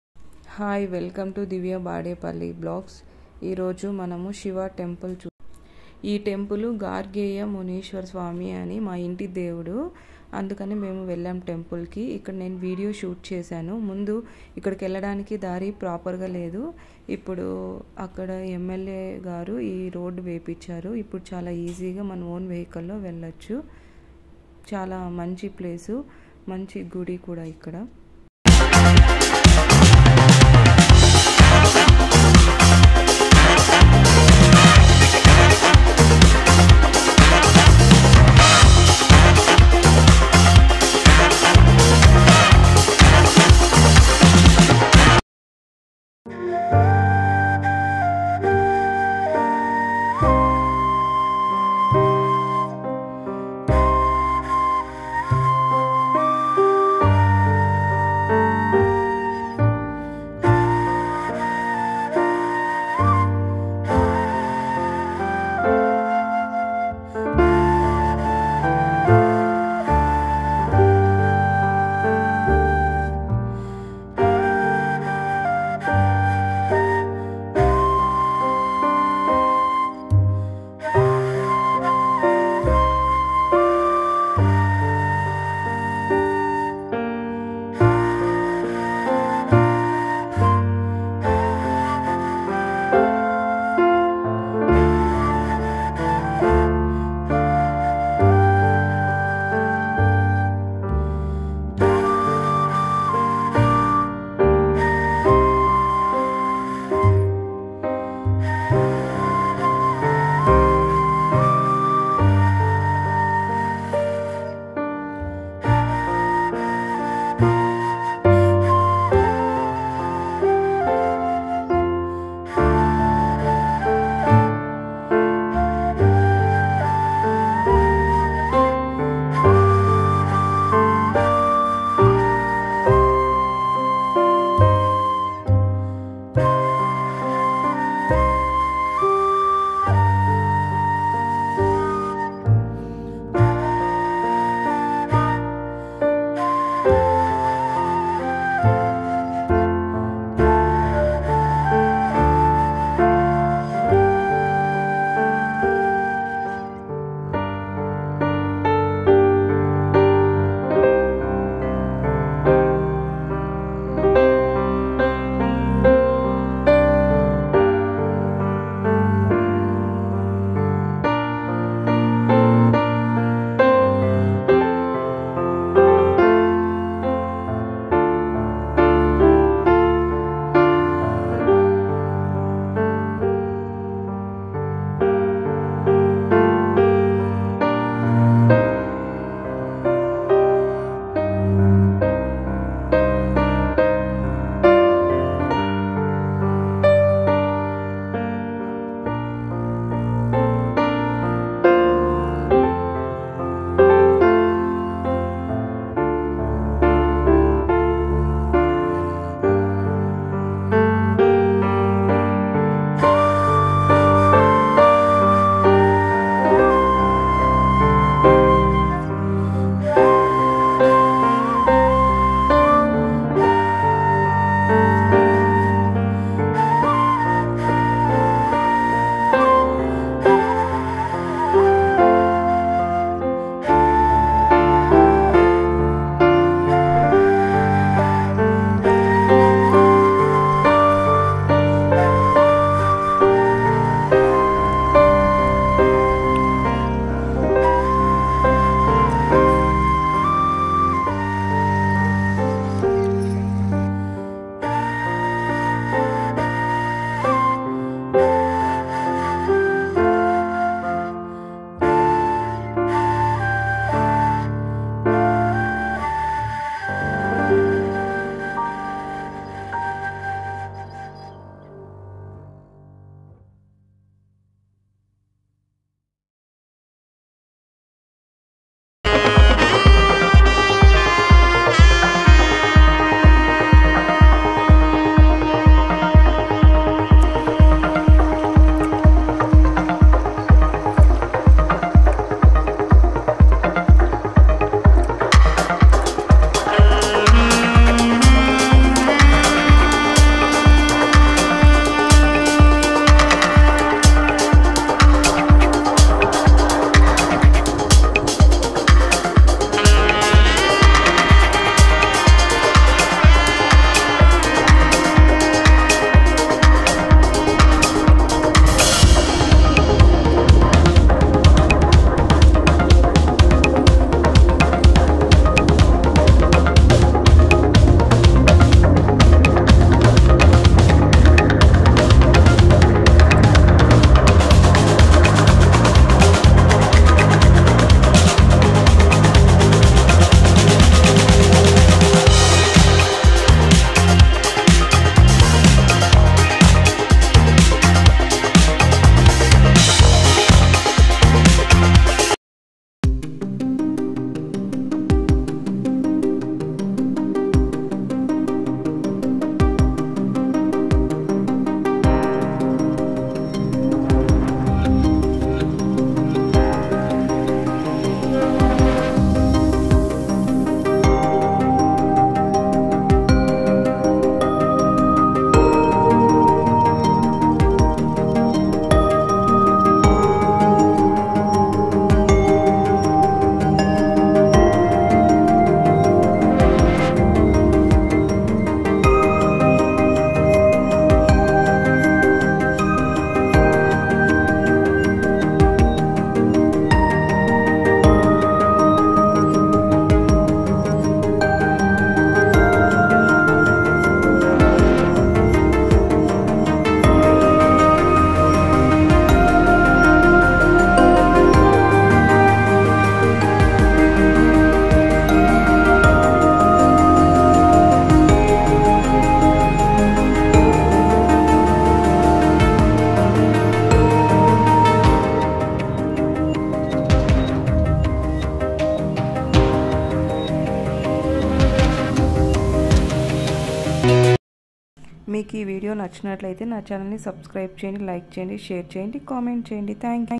శ్రీ గార్గేయ మునీశ్వర స్వామి దేవాలయం/God Shiva temple, famous temple in chittoor district
శ్రీ గార్గేయ మునీశ్వర స్వామి దేవాలయం,
దుర్గం, పెద్ద ఉప్పరపల్లి,
సోమల(ఎం), చిత్తూరు (డిస్), AP.

Monday & festival time poojari is available the remaining days we will call him, he will come.

sri gargeya swamy temple/shiva temple .
ముందు గుడి కి వెళ్ళడానికి చాల కష్టంగా ఉంది,ఇప్పుడు రోడ్డు చాల బాగా వేసారు ఈజీ గా వెళ్లి దేవుడిని దర్శనం చేసుకోవచ్చు, గుడి కింద నిర్మాణం చేస్తున్నారు

My channel
Divya Badepalli vlogs
Thankyou 😍